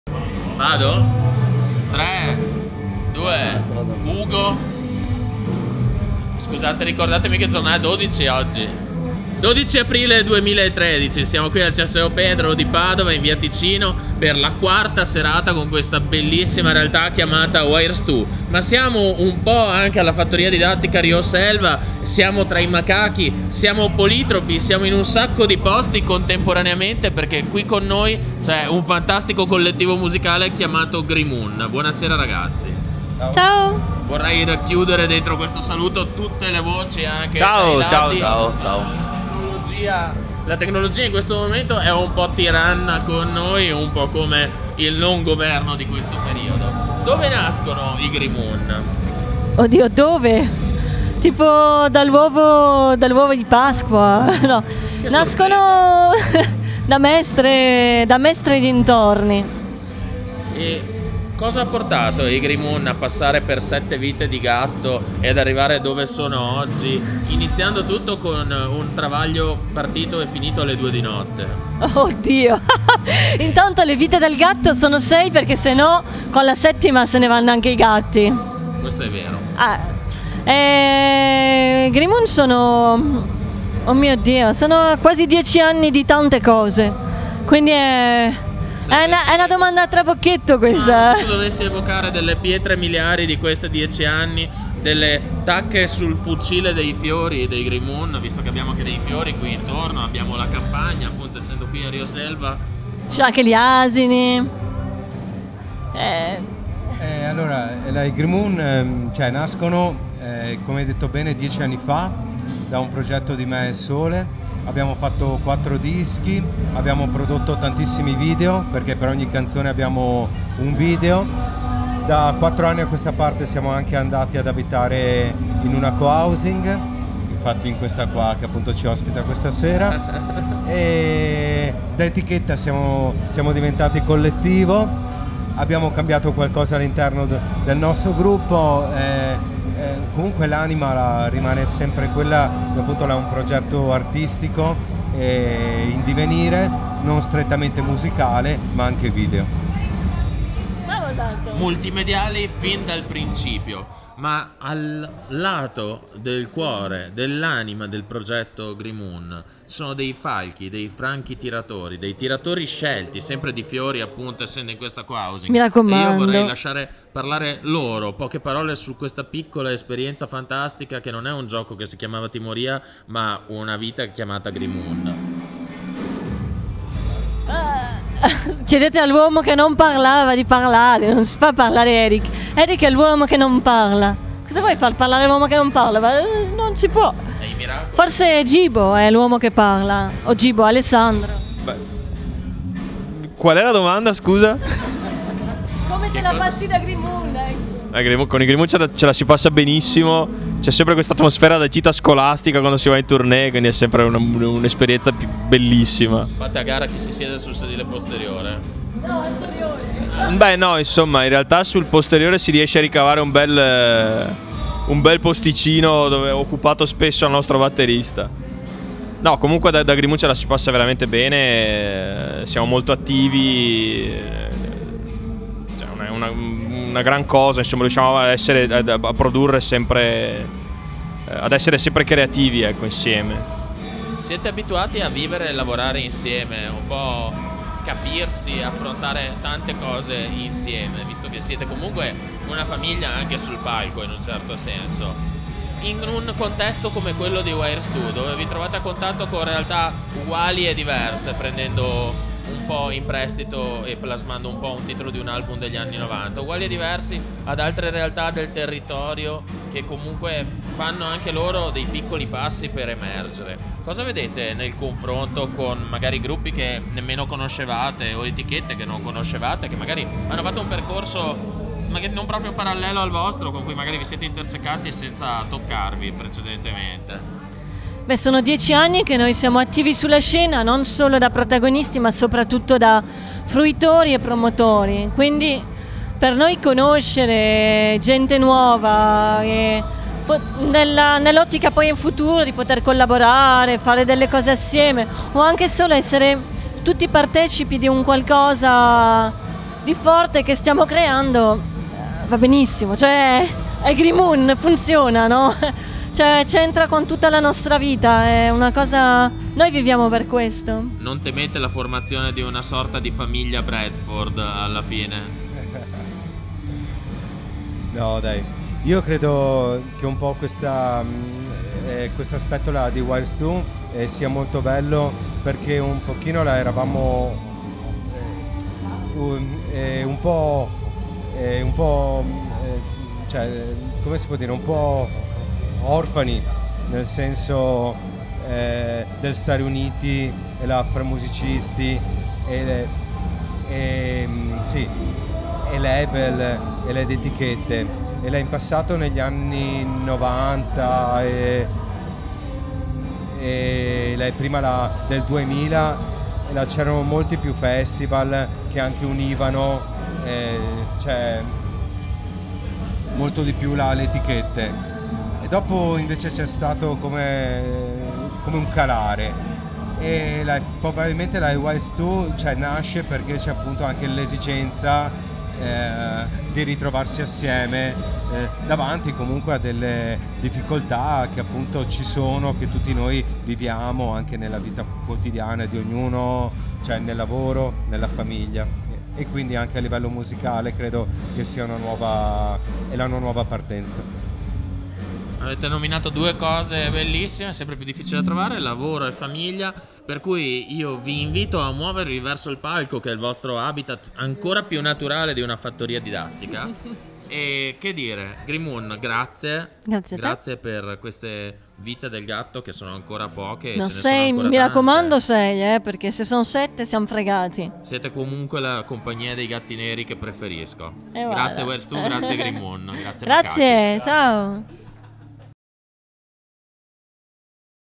Intervista Grimoon
Venerdì 12 Aprile 2013 - C.S.O. Pedro - Wires to #5 - Intervista ai Grimoon